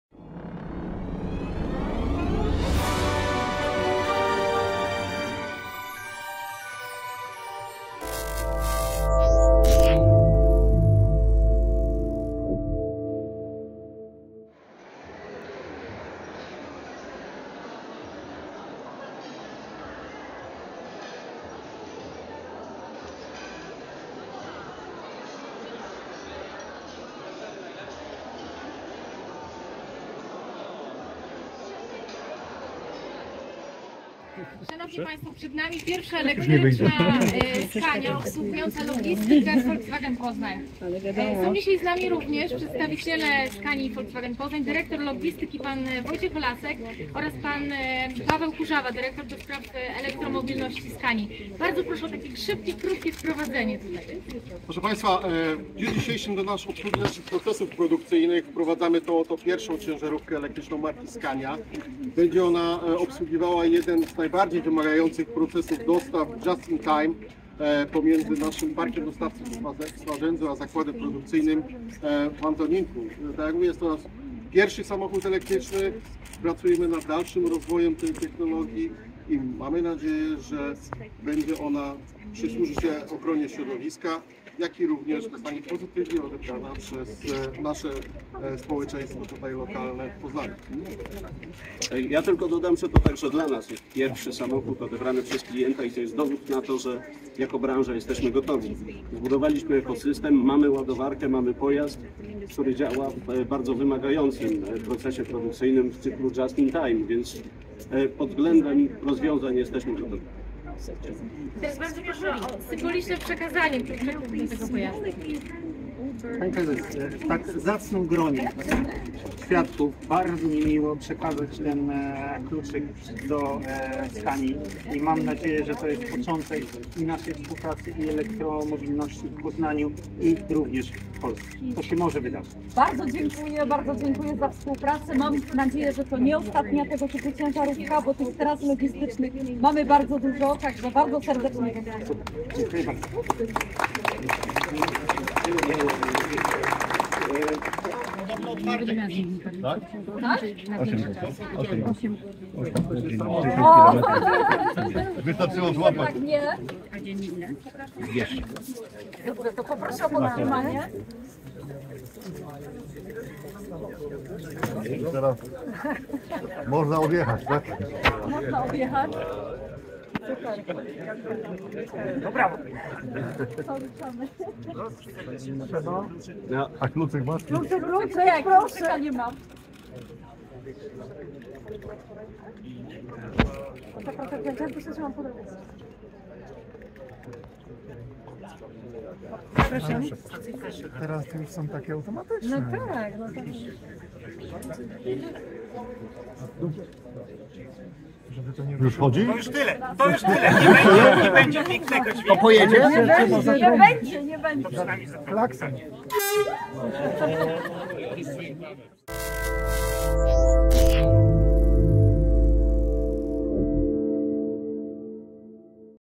Scania 25P B6x2*4NB Elektryczny pojazd ciężarowy Poznań Motor Show 2023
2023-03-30
W pełni elektryczny pojazd ciężarowy Scania 25P będzie wykorzystywany w procesie codziennych dostaw w systemie just-in-time do fabryki Volkswagen Poznań w Antoninku. Przewożony ładunek, to podzespoły produkowanego tam Volkswagena Caddy, a dokładnie jego tylne osie. Co ważne, jego wykorzystanie zaplanowano w systemie trzyzmianowym. Na potrzeby ładowania tego i przyszłych elektrycznych pojazdów wykorzystywanych przez Logistykę Volkswagen Poznań, na terenie zakładu Volkswagen Poznań powstała stacja ładowania aut zieloną energią o mocy 120 kW. Na takiej stacji można naładować samochód „do pełna” w niecałe 40 minut.
Scania 25P B6x2*4NB to elektryczny pojazd ciężarowy, przeznaczony do realizacji zadań typowych dla dystrybucji miejskiej. Jego najważniejsze zalety, to m.in. zerowa emisja, bardzo niski poziom generowanego hałasu. Samochód wyposażony jest w pięć baterii o łącznej pojemności 165 kWh, pozwalającej – w zależności od warunków – na ok. 8 godzin pracy. Parametry pojazdu zostały w pełni dostosowane do charakteru wykonywanych zadań transportowych oraz pokonywanej trasy.